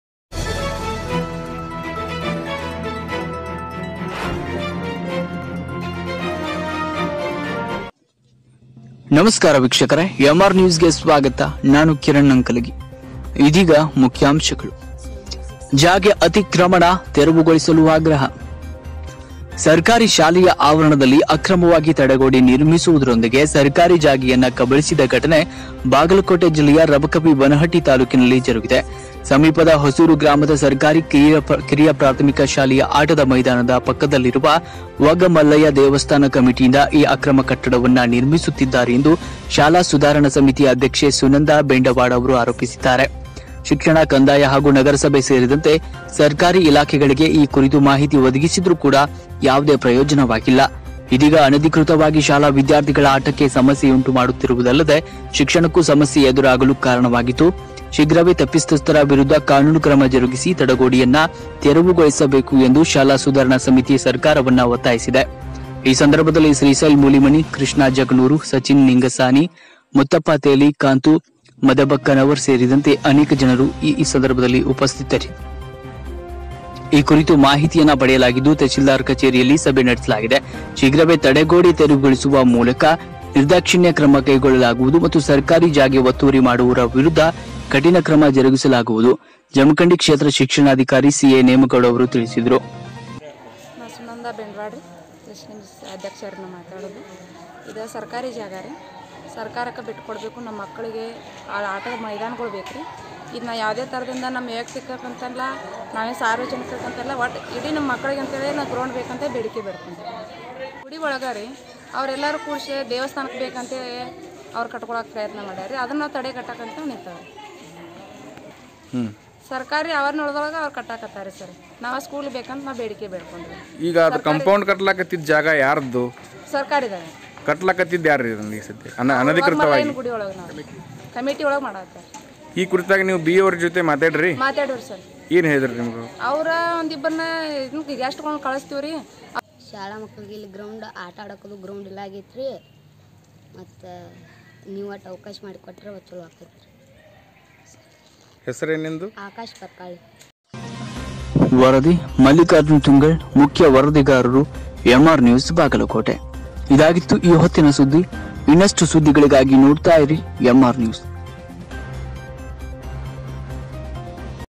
ಜಾಗೆ ಅತಿಕ್ರಮಣ: ತೆರವುಗೊಳಿಸಲು ಆಗ್ರಹ.
ಸರ್ಕಾರಿ ಶಾಲೆಯ ಆವರಣದಲ್ಲಿ ಅಕ್ರಮವಾಗಿ ತಡೆಗೋಡೆ ನಿರ್ಮಿಸುವದರೊಂದಿಗೆ ಸರ್ಕಾರಿ ಜಾಗೆಯನ್ನೇ ಕಬಳಿಸಿದ ಘಟನೆ ಬಾಗಲಕೋಟೆ ಜಿಲ್ಲೆಯ ರಬಕವಿ-ಬನಹಟ್ಟಿ ತಾಲೂಕಿನಲ್ಲಿ ಜರುಗಿದೆ.ಸಮೀಪದ ಹೊಸೂರ ಗ್ರಾಮದ ಸರ್ಕಾರಿ ಕಿರಿಯ ಪ್ರಾಥಮಿಕ ಶಾಲೆಯ ಆಟದ ಮೈದಾನದಲ್ಲಿ ಪಕ್ಕದಲ್ಲಿಯಿರುವ ವಗ್ಗ ಮಲ್ಲಯ್ಯ ದೇವಸ್ಥಾನ ಕಮಿಟಿಯಿಂದ ಈ ಅಕ್ರಮ ಕಟ್ಟಡವನ್ನು ನಿರ್ಮಿಸುತ್ತಿದ್ದಾರೆಂದು ಶಾಲಾ ಸುಧಾರಣಾ ಸಮಿತಿ ಅಧ್ಯಕ್ಷೆ ಸುನಂದಾ ಬೆಂಡವಾಡ ಆರೋಪಿಸಿದ್ದಾರೆ.

ಶಿಕ್ಷಣ, ಕಂದಾಯ ಹಾಗು ನಗರಸಭೆ ಸೇರಿದಂತೆ ಸರ್ಕಾರಿ ಇಲಾಖೆಗಳಿಗೆ ಈ ಕುರಿತು ಮಾಹಿತಿ ಒದಗಿಸಿದರೂ ಯಾವದೇ ಪ್ರಯೋಜನವಾಗಿಲ್ಲ. ಇದೀಗ ಅನಧಿಕೃತವಾಗಿ ಶಾಲಾ ವಿದ್ಯಾರ್ಥಿಗಳ ಆಟಕ್ಕೆ ಸಮಸ್ಯೆಯುಂಟು ಮಾಡುತ್ತಿರುವದಲ್ಲದೆ ಶಿಕ್ಷಣಕ್ಕೂ ಸಮಸ್ಯೆ ಎದುರಾಗಲು ಕಾರಣವಾಗಿದ್ದು, ಶೀಘ್ರವೇ ತಪ್ಪಿತಸ್ಥರ ವಿರುದ್ಧ ಕಾನೂನು ಕ್ರಮ ಜರುಗಿಸಿ, ತಡೆಗೋಡೆಯನ್ನು ತೆರವುಗೊಳಿಸಬೇಕೆಂದು ಶಾಲಾ ಸುಧಾರಣಾ ಸಮಿತಿ ಸರ್ಕಾರವನ್ನು ಒತ್ತಾಯಿಸಿದೆ.

ಈ ಸಂದರ್ಭ ಶ್ರೀಶೈಲ ಮೂಲಿಮನಿ, ಕೃಷ್ಣಾ ಜಕನೂರ, ಸಚಿನ್ ನಿಂಗಸಾನಿ, ಮುತ್ತಪ್ಪ ತೇಲಿ, ಕಾಂತು ಮದಬಕ್ಕನವರ ಸೇರಿದಂತೆ ಅನೇಕರಿದ್ದರು.

`ಈ ಕುರಿತು ಮಾಹಿತಿ ಪಡೆಯಲಾಗಿದ್ದು, ತಹಶೀಲ್ದಾರ ಕಚೇರಿಯಲ್ಲಿ ಸಭೆ ನಡೆಸಲಾಗಿದೆ. ಶೀಘ್ರವೇ ತಡೆಗೋಡೆ ತೆರವುಗೊಳಿಸುವ ಮೂಲಕ ನಿರ್ದಾಕ್ಷಿಣ್ಯ ಕ್ರಮ ಕೈಗೊಳ್ಳಲಾಗುವದು ಮತ್ತು ಸರ್ಕಾರಿ ಜಾಗೆ ಒತ್ತುವರಿ ಮಾಡುವವರ ವಿರುದ್ಧ ಕಠಿಣ ಕ್ರಮ ಜರುಗಿಸಲಾಗುವದು.ಜಮಖಂಡಿ ಕ್ಷೇತ್ರ ಶಿಕ್ಷಣಾಧಿಕಾರಿ ಸಿ.ಎ. ನೇಮಗೌಡ ತಿಳಿಸಿದರು.

ವರದಿ
ಮಲ್ಲಿಕಾರ್ಜುನ ತುಂಗಳ
ಮುಖ್ಯ ವರದಿಗಾರರು
ಬಾಗಲಕೋಟೆ